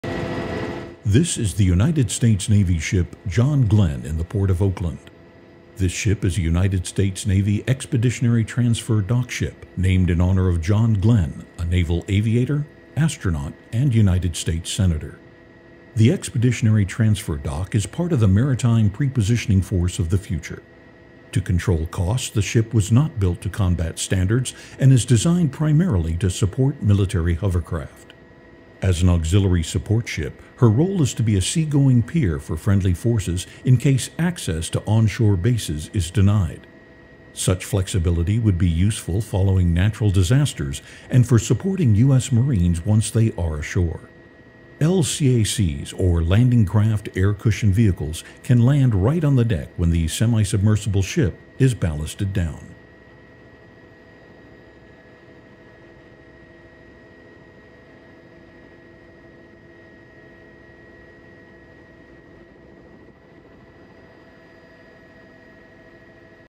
USNS John Glenn
The Expeditionary Transfer Dock is a new concept, part of the Maritime Prepositioning Force of the future. To control costs, the ships will not be built to combat vessel standards and are designed primarily to support three military hovercraft (such as the Landing Craft Air Cushion (LCAC)), vehicle staging with a sideport ramp and large mooring fenders. A decision was made to eliminate helicopter capability and ship-to-ship transfer of heavy equipment

Class and type Montford Point-class expeditionary transfer dock
Displacement 34,500 tonnes
Length 765 ft (233 m)
Speed 20 knots (37 km/h; 23 mph)
Range 9,000 nmi (17,000 km; 10,000 mi)